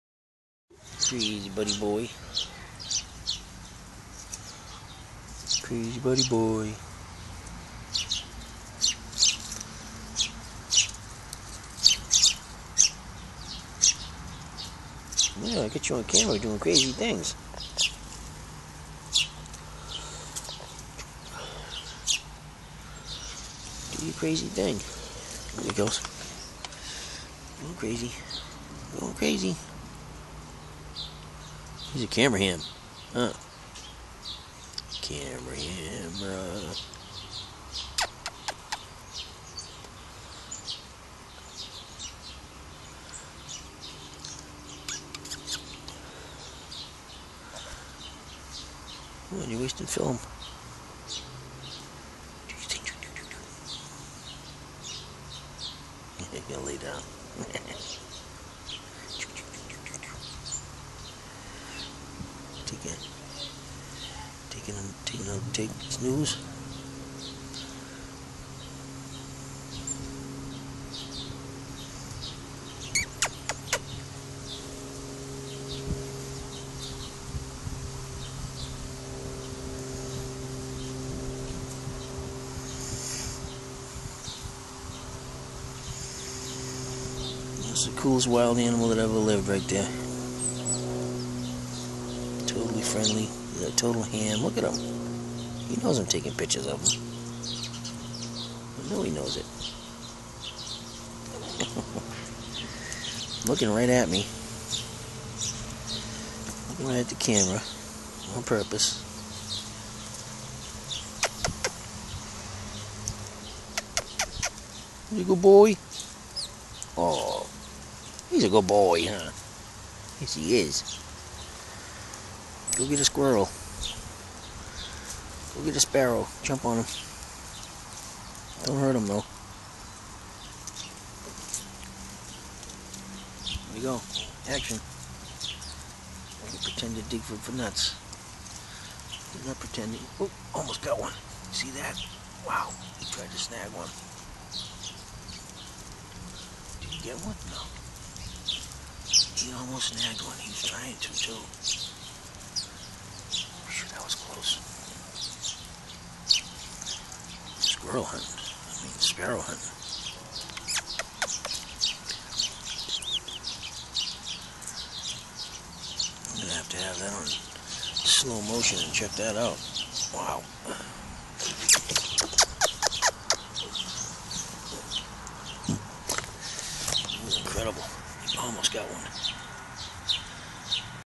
Squirrel hunting sparrows
Squirrel almost snags sparrow